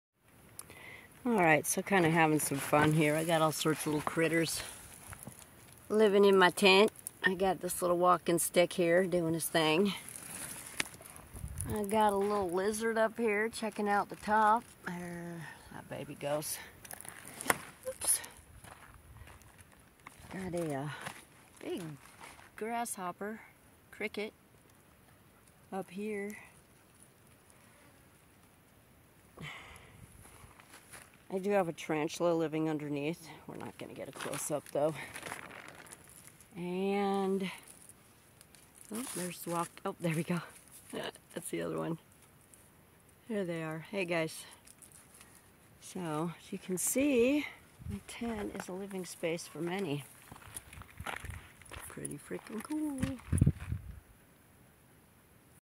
my tent becoming a community
if you're interested in coming down to the Arizona Mexican border wall and helping out we have children being traffic through and could use boots on the ground..